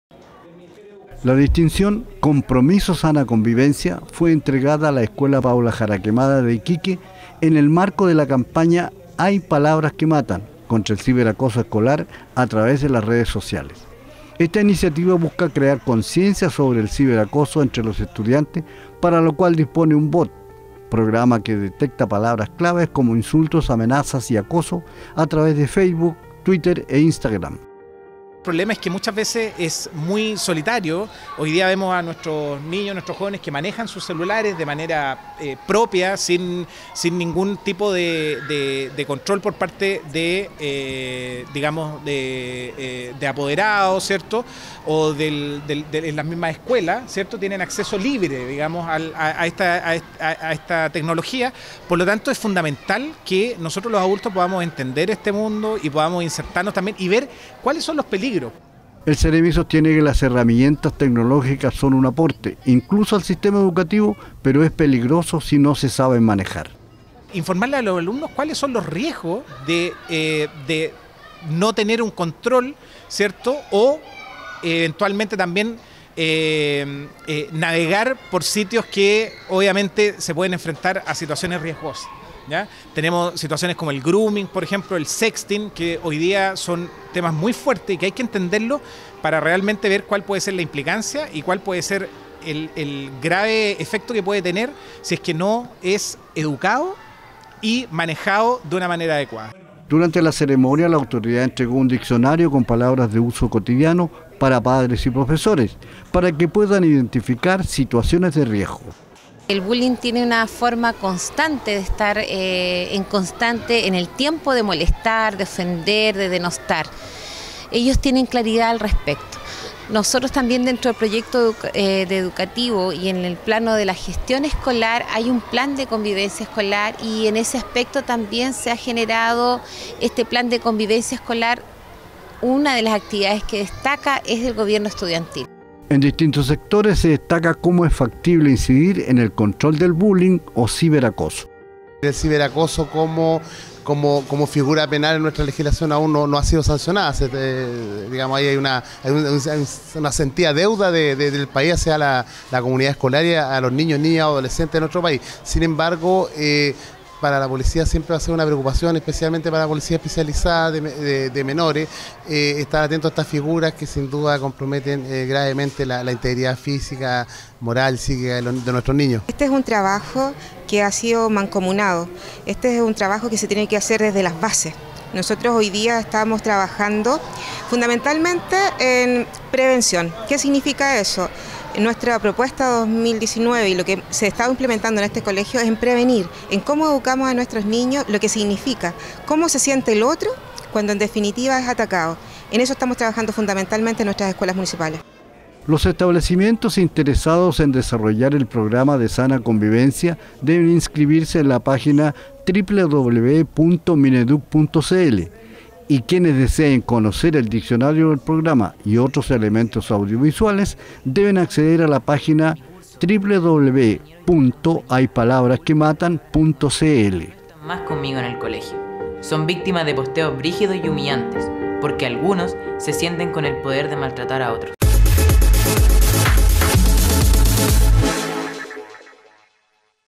Campaña “Hay palabras que matan“ combate ciber acoso (2018.11.06) Iquique TV
La distinción “Compromiso Sana Convivencia”  fue entregada a la Escuela Paula Jaraquemada de Iquique,  en el marco del inicio de la campaña “Hay palabras que matan”, actividad con la que autoridades de la educación dieron inicio en Tarapacá  a las acciones del sector educación contra el ciber acoso escolar.
Esta iniciativa, busca crear conciencia sobre el ciber acoso en estudiantes, para lo cual dispone un bot, programa que protege a la comunidad escolar de episodios de violencia, al detectar palabras claves como insultos, amenazas y acoso  a través de Facebook, Twitter e Instagram, en el momento de su ocurrencia.